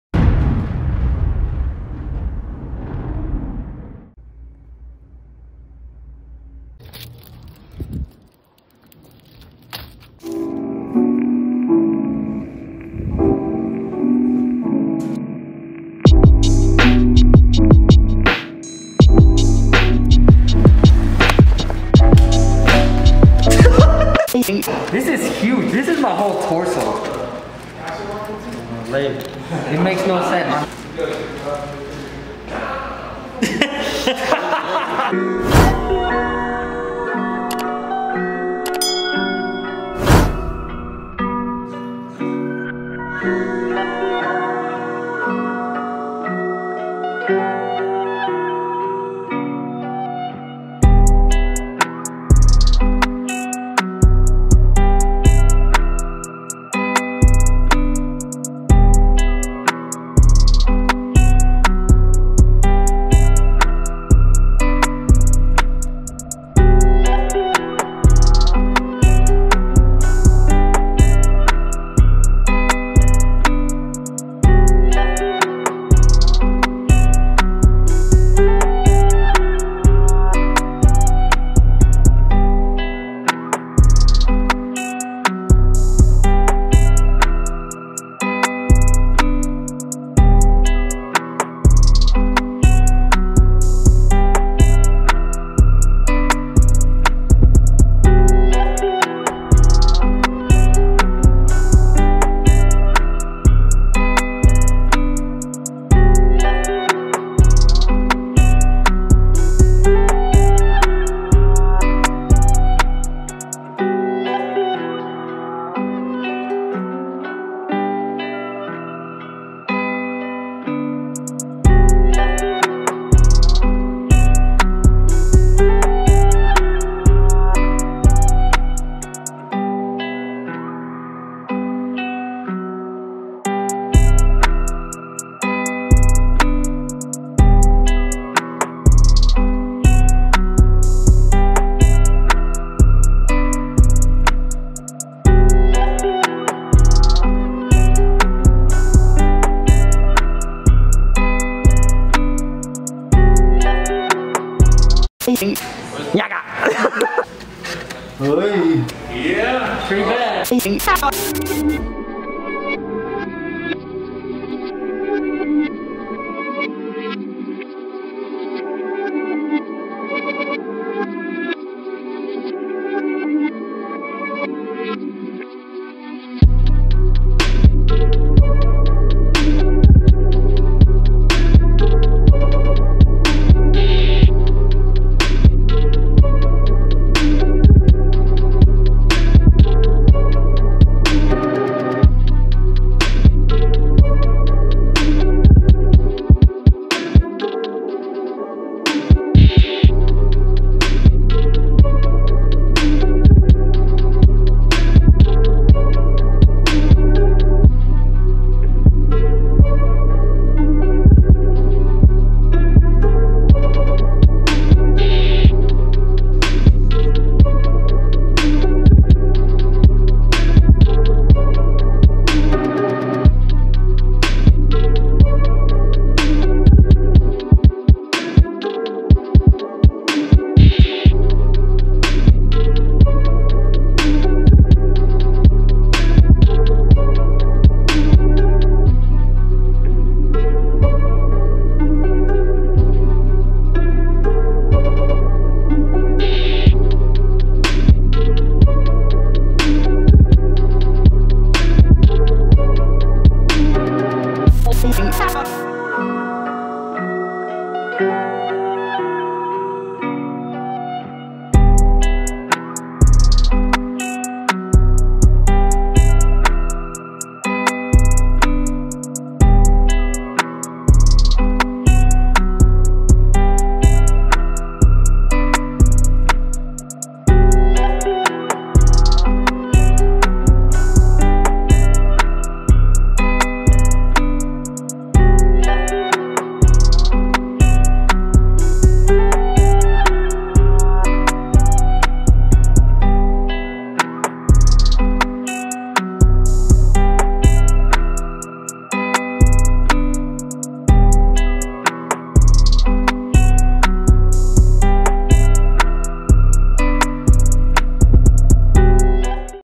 OUTER LEG SLEEVE COMPLETED IN ONE DAY ✖️roses +portrait ✖️🔸TATTOO TIME LAPSE (BY MR.REYES_INK)
Tattoo timelapse: tattoo tutorial with a lot of real time tattooing process from start to finish . i hope you find this video useful and you learned a new trick or maybe have a better understanding on how a tattoo is applied if you're new to the industry. thank you for watching and see you on the next video.🤘🏽

🔸Specializing in Large scale tattoo Black and Grey/Cover-ups
Phoenix Az🌵

🔸Recording equipment:
✖️canon rebel sl2

✖️iPhone max pro

✖️Ipad pro